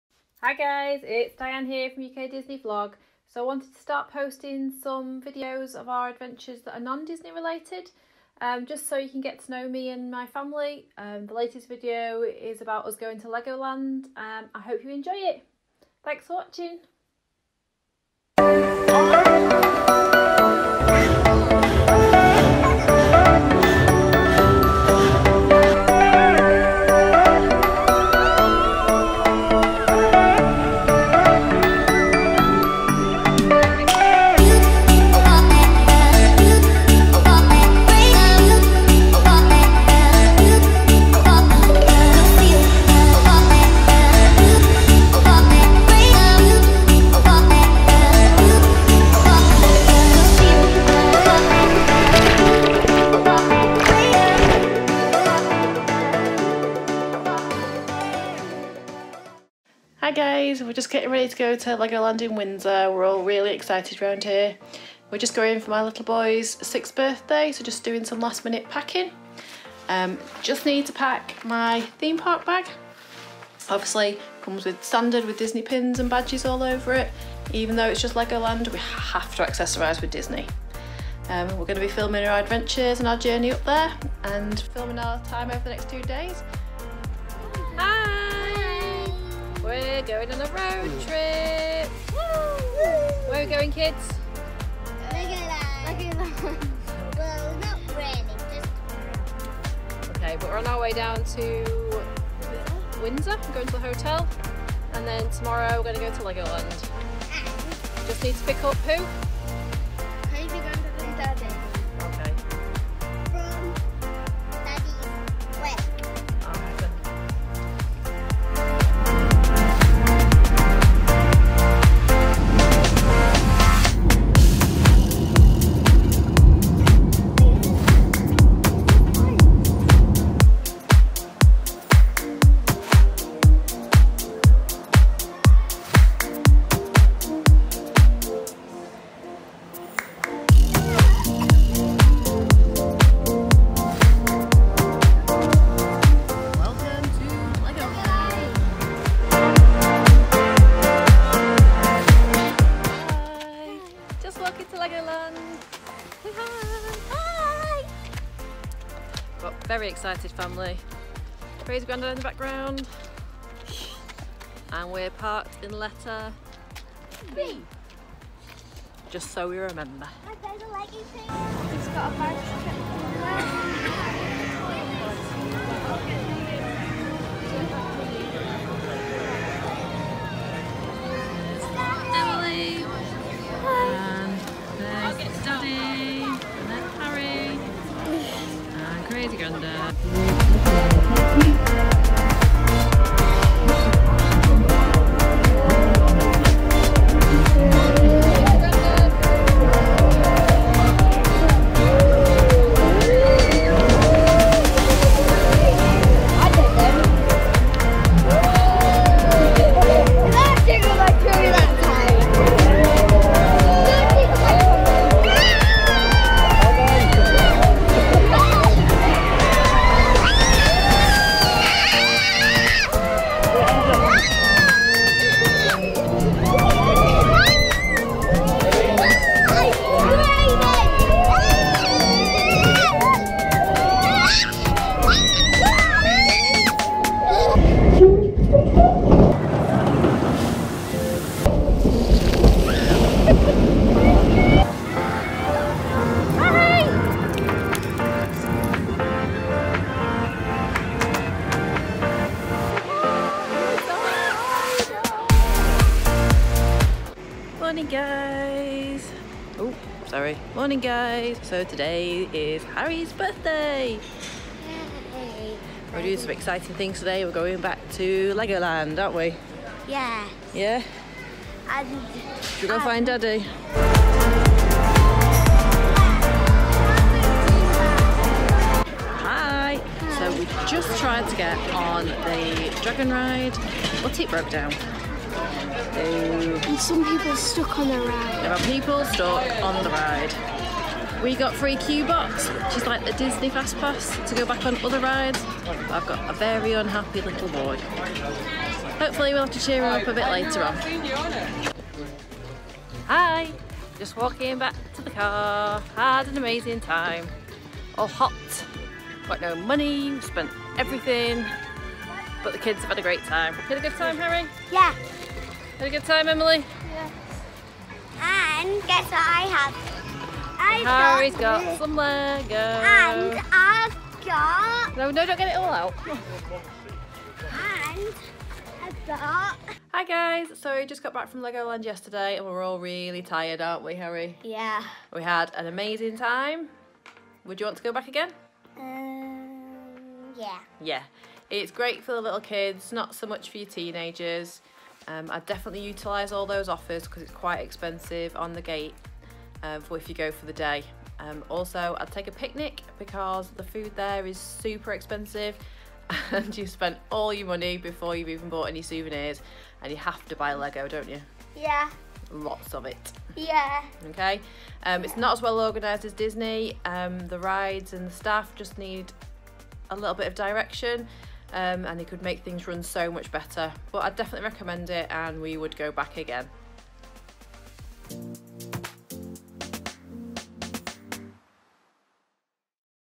Legoland Windsor Trip
Short vlog about our Family trip to Legoland Windsor